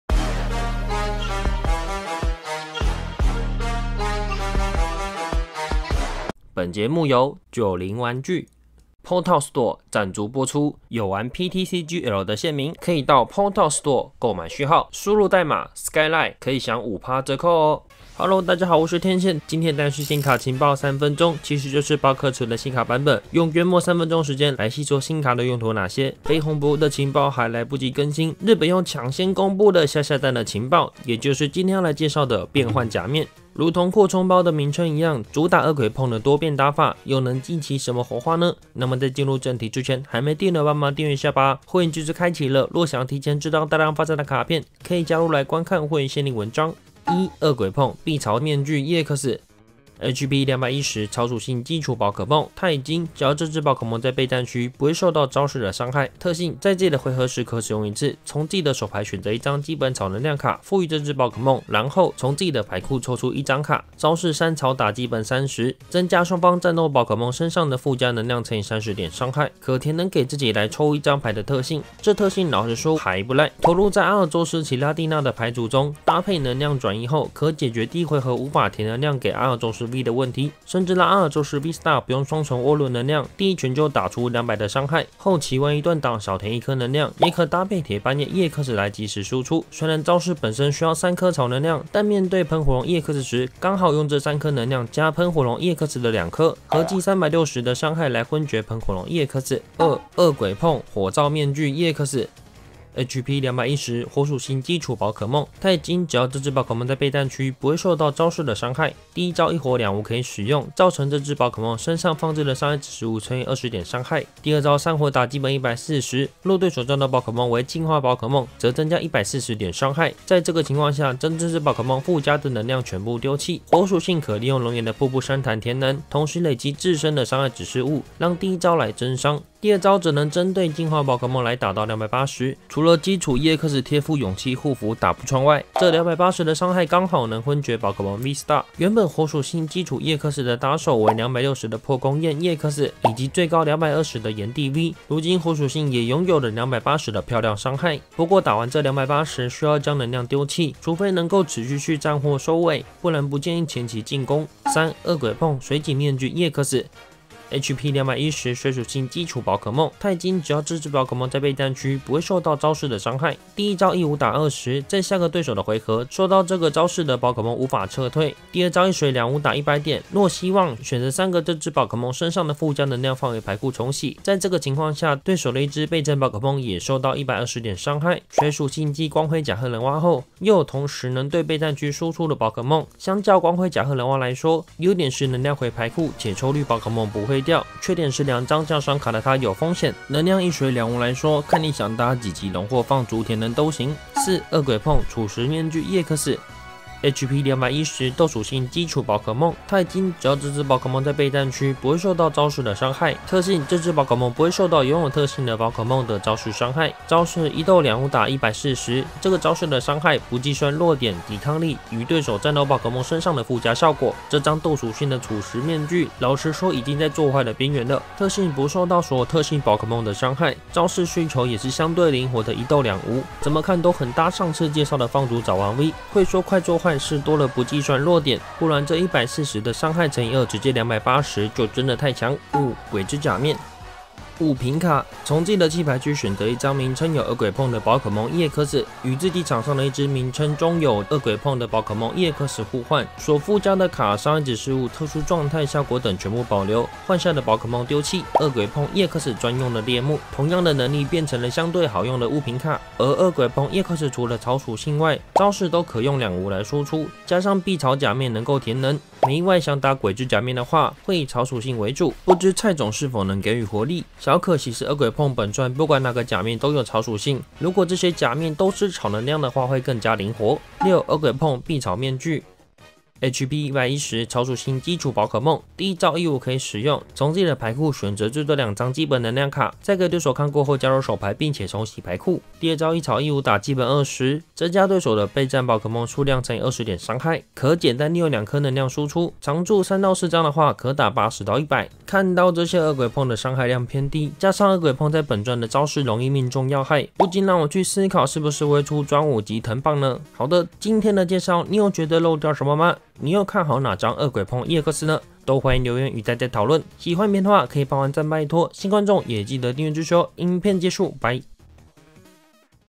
【新卡情報3分鐘】像本傳一樣能換裝的厄鬼碰ex!? 變幻假面｜寶可夢卡牌 PTCG｜天線 SkyLine
用線上版練牌卻鬧序號飢荒嗎?
趕快到PoTown Store
輸入序號可享5%折扣喔：Skyline